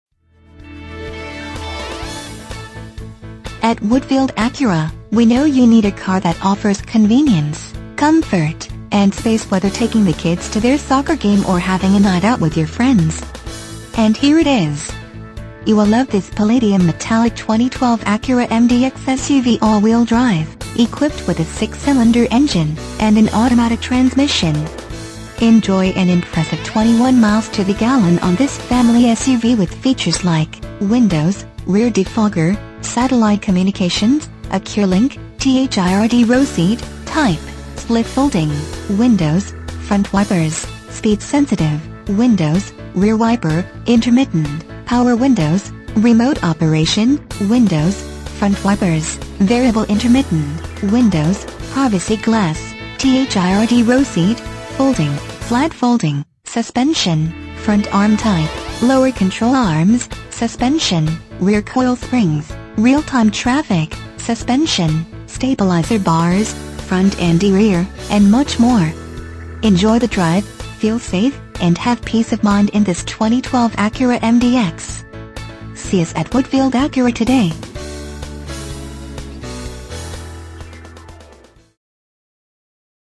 2012 Acura MDX Woodfield Acura Hoffman Estates, IL 60195
Acura MDX At Woodfield Acura, we know you are looking for a vehicle to solve everyday task. Whether loading the kids soccer equipment or having a night out with your friends. You will have the room you need making those everyday task an ease to accomplish. Imagine driving this Palladium Metallic Acura Mdx SUV AWD, equipped with a 6 Cylinder engine and an automatic transmission.
MILES: 36,918
VIN:2HNYD2H30CH516077

Woodfield Acura
1099 w. Higgins Rd.